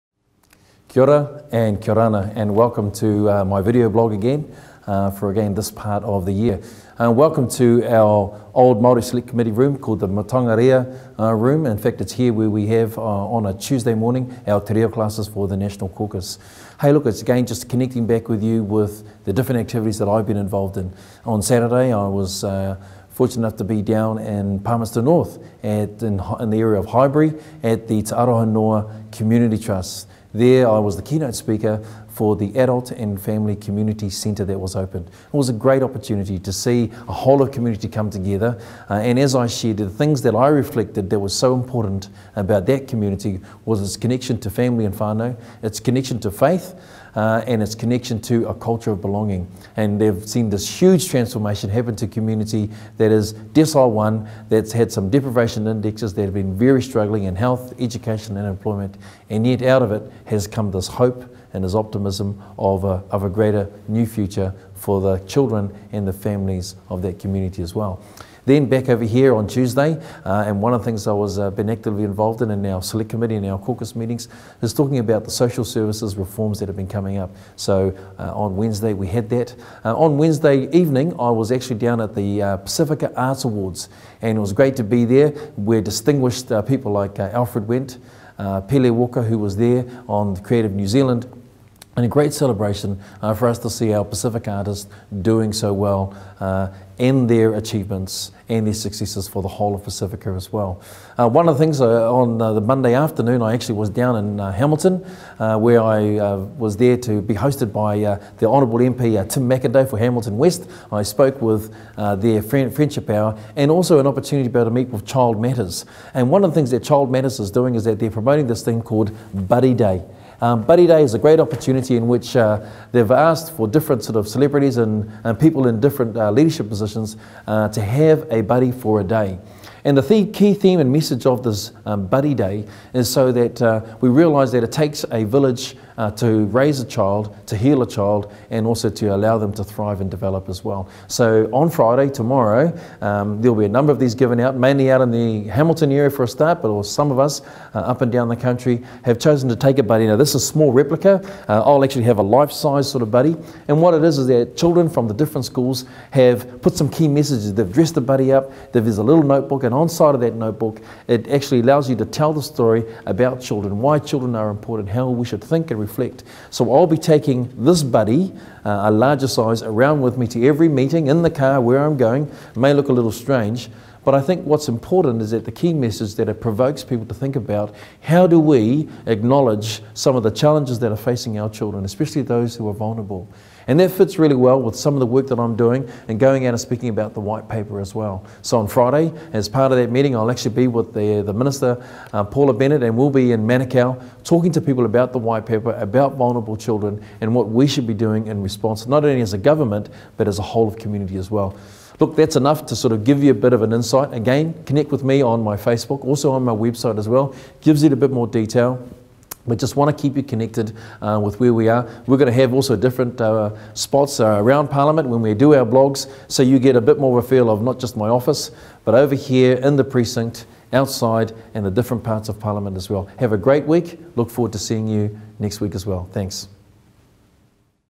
Alfred Ngaro MP - Video Update
Alfred talks about his recent involvement with the opening of a new adult and family community centre in Palmerston North, the Pacifica Arts Awards and the Buddy Day initiative